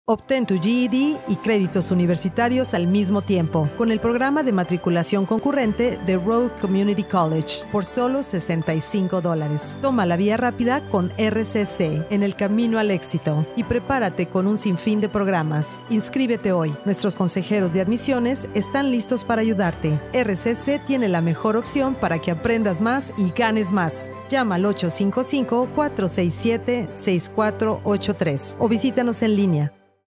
Obtén tu GED y créditos universitarios en RCC (30 sec)
Obtén tu GED y créditos universitarios al mismo tiempo con el programa de matriculación concurrente de RCC por solo $65. ¡Así es, $65! Esta es la vía rápida en el camino a prepararte para el éxito en un sinfín de programas de RCC. RCC tiene realmente un camino para que aprendas más y tengas más ingresos. ¡Inscríbete hoy!